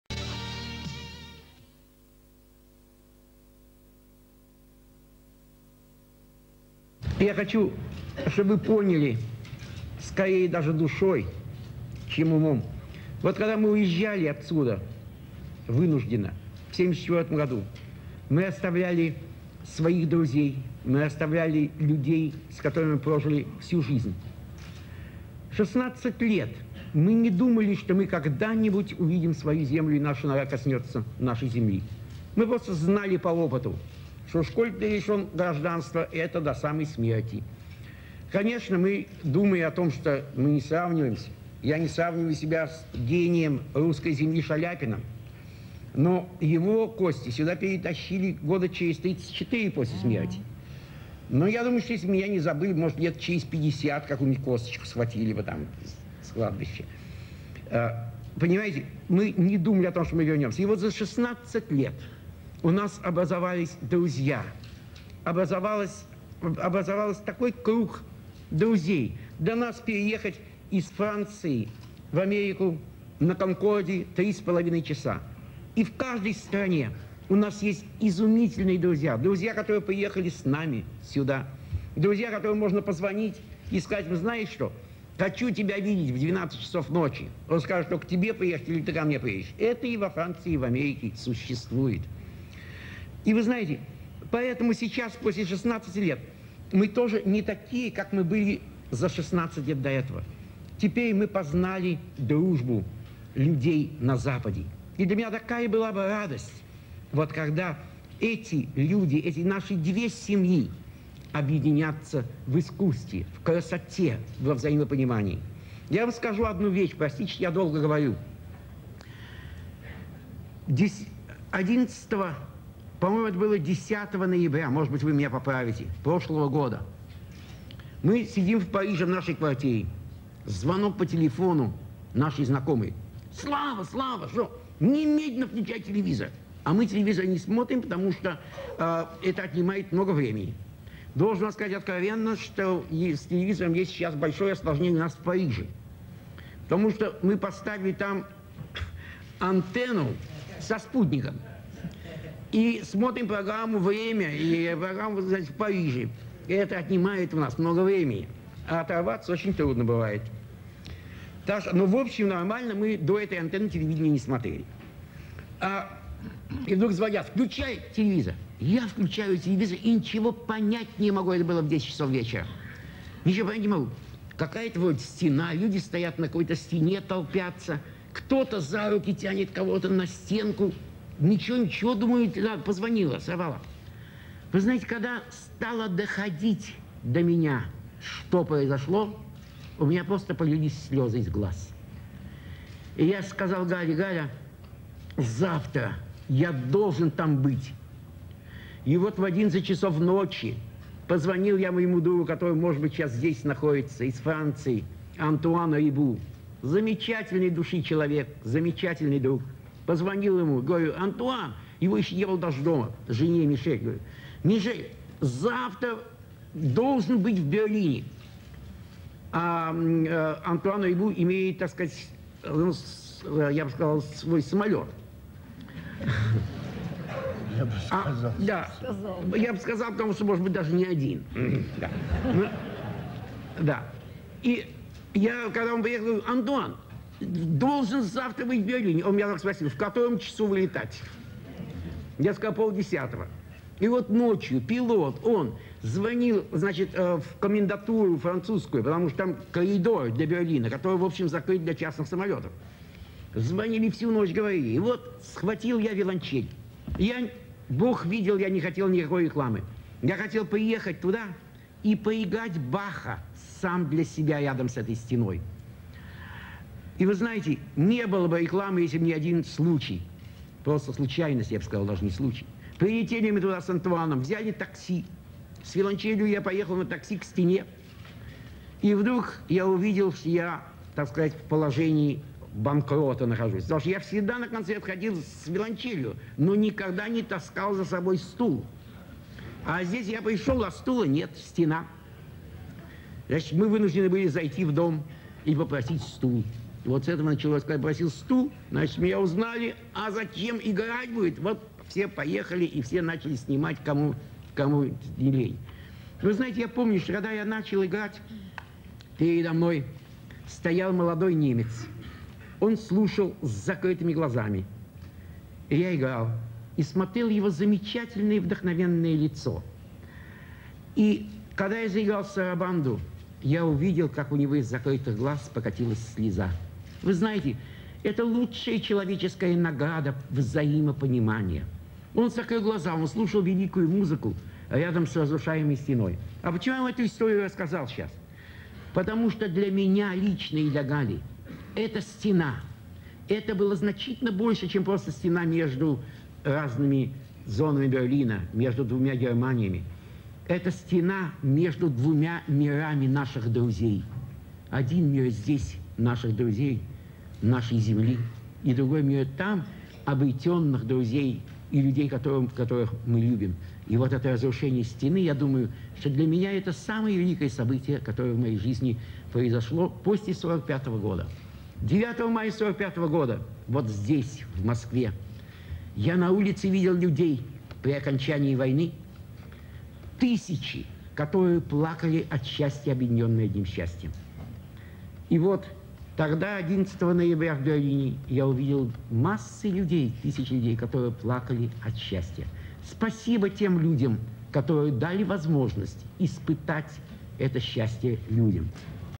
Press Conference ~ Mstislav Rostropovich 3/6
Press Conference
Mstislav Rostropovich
Galina Vishnevskaya
Nikolai Gubenko
February 8, 1990
Пресс - конференция
Мстислав Ростропович
Галина Вишневская
Николай Губенко
08 февраля 1990 года